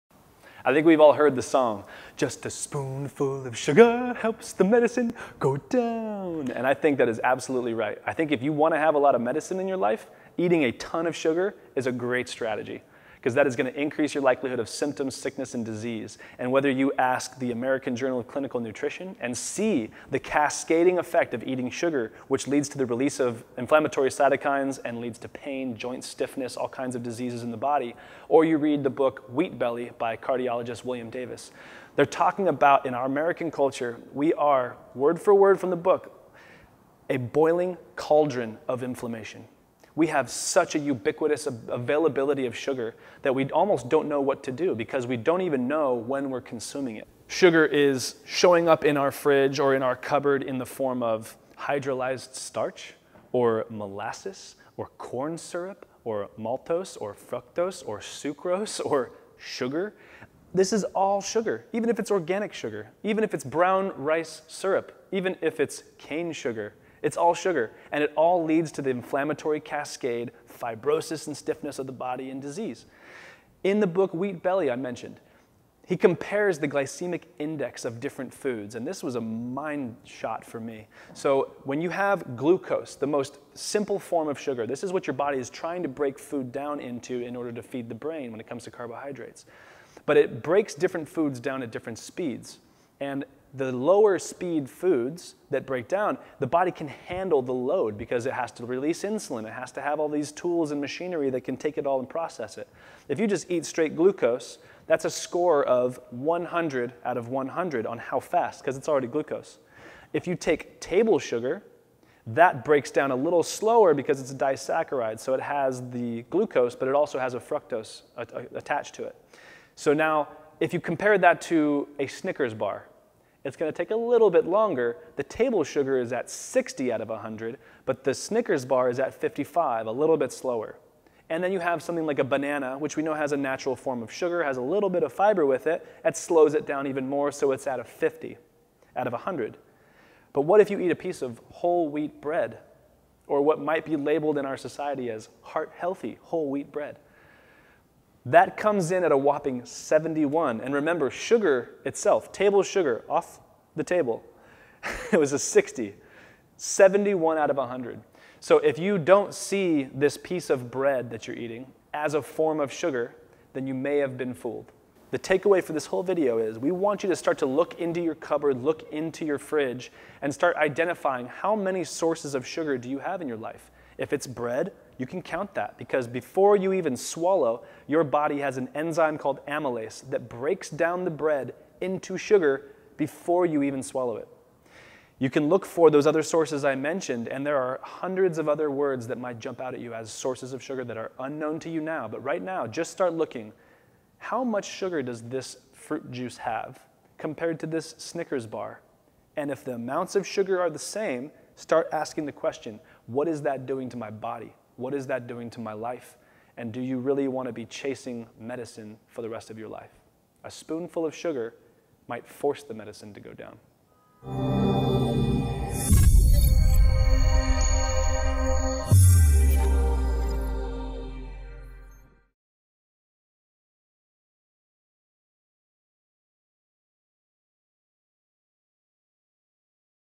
How Sugar Leads to Pain and Inflammation - The Source Chiropractic
Dr. Lance von Stade breaks down how consuming sugar leads to inflammation throughout the body, pain, symptoms, stiffness and disease.
How sugar is hidden with many different names
And asks the question, how much sugar are you consuming, and how is that sugar effecting my life?